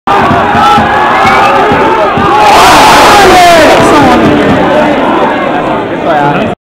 Werder Bremen U23 : Dresden 1:2 (29.08.2015)
Ousman Manneh mit der Chance zum 2:0 (67.)